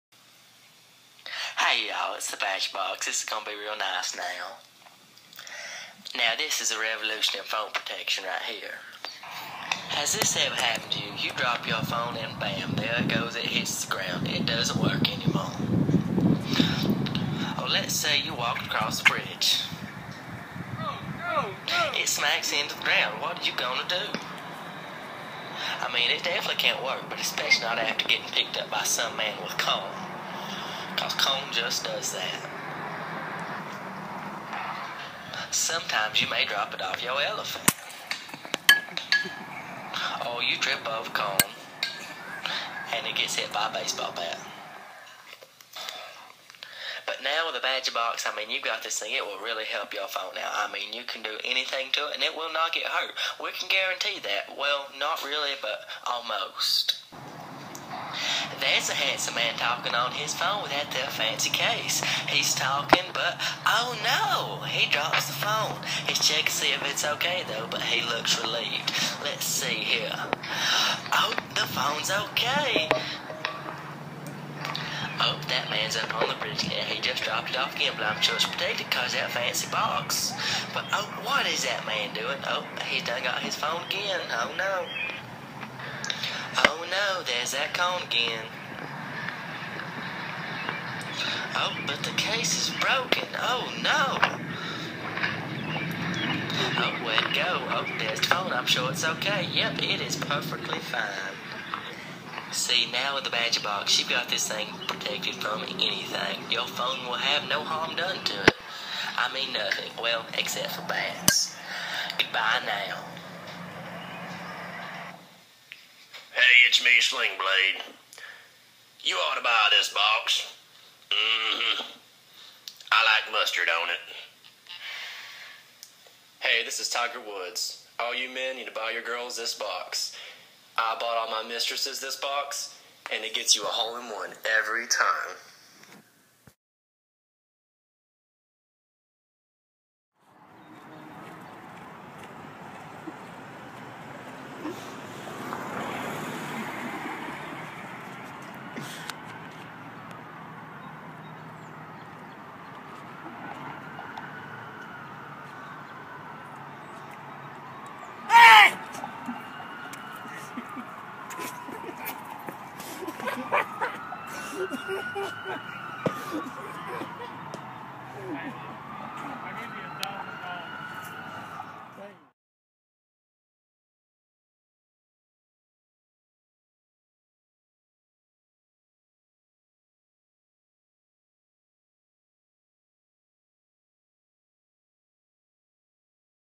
The Badger Box Commercial
Drew, Tyson, Nathan, and Dray make a video advertising the best phone case ever known to man.